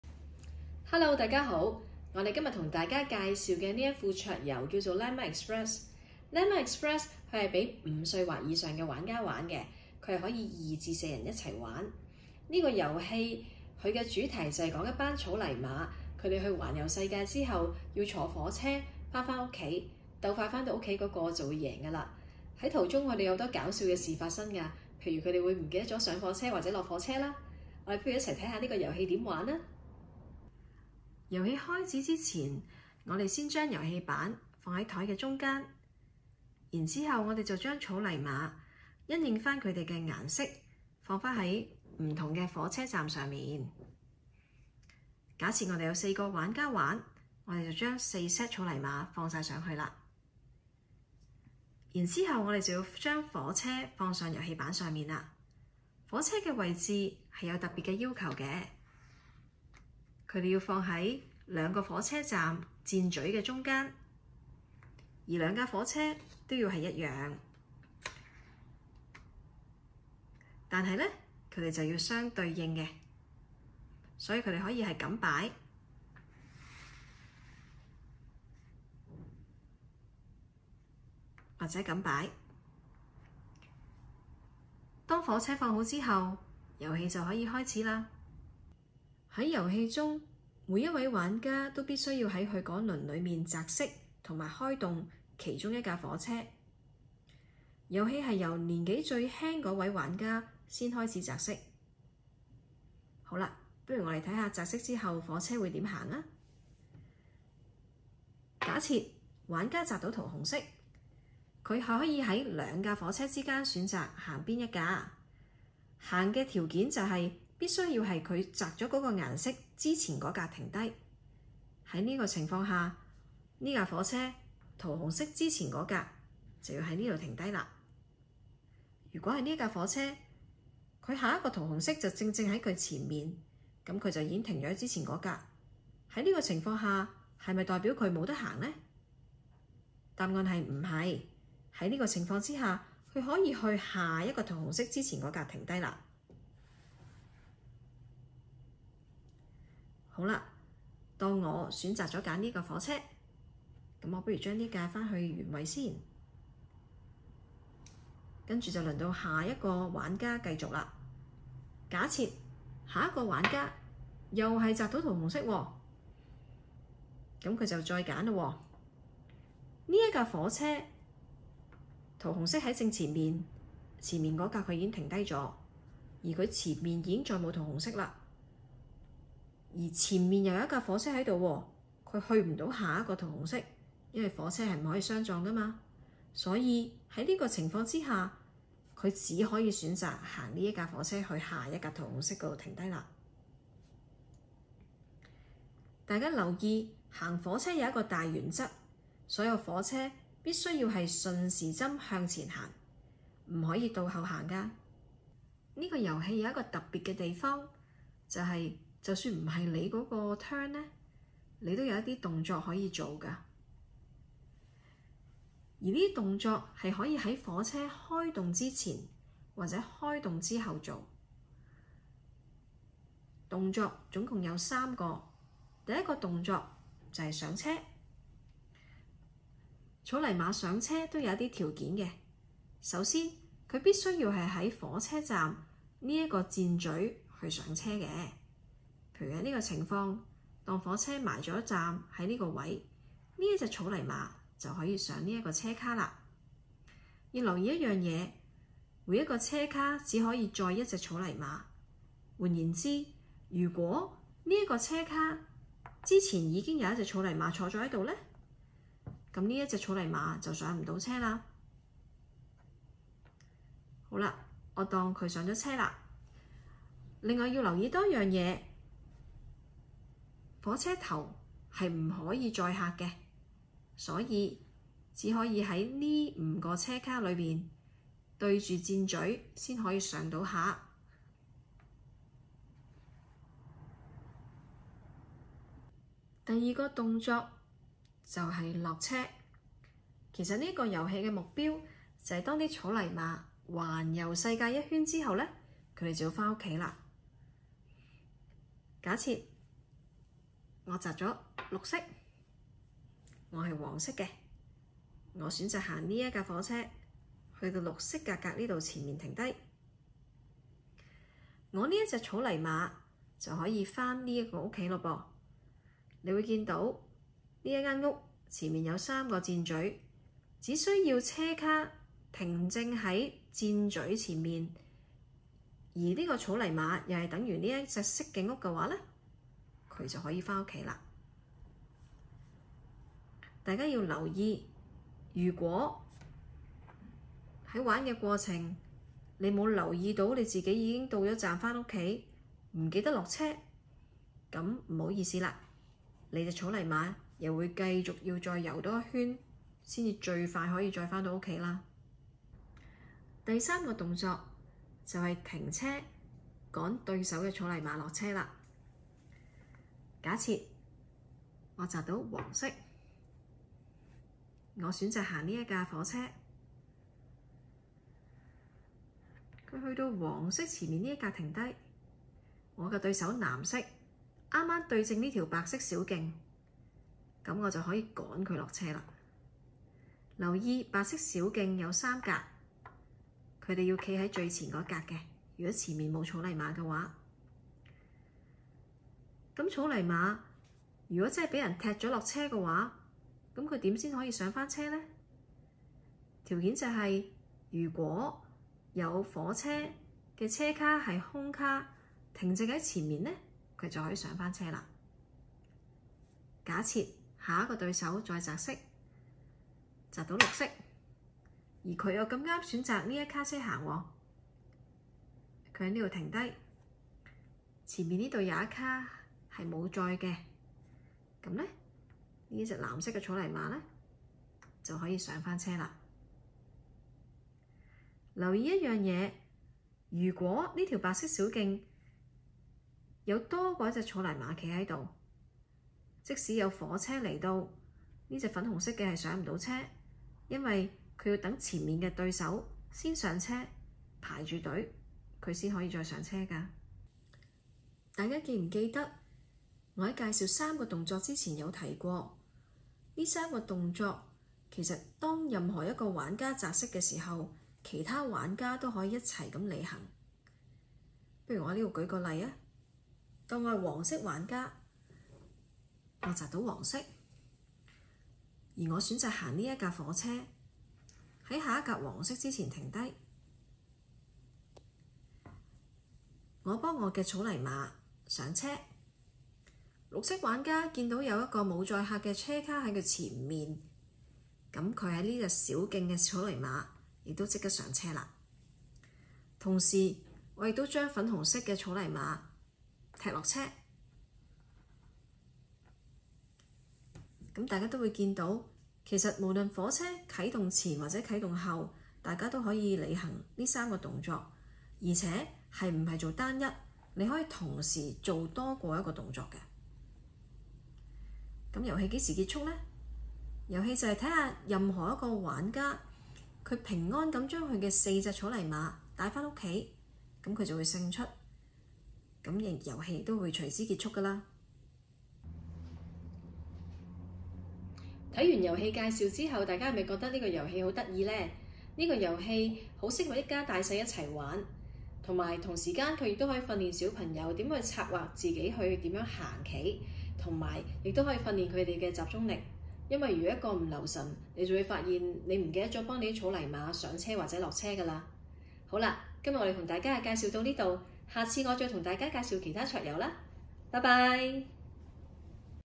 Lama Express 桌遊教學
Lama Express 帶你環遊世界！當你的草泥馬登上Lama Express後，牠們會穿越歐洲、美洲、亞洲及非洲四個大陸。各玩家要在遊戲版上讓自己的草泥馬環遊各大洲之餘，所有草泥馬首先回到家的便可勝出遊戲，千萬不要讓草泥馬忘記到站下火車啊！
- 玩家人數: 2-4
- 建議年齡: 5歲或以上
- 遊戲時間: 25分鐘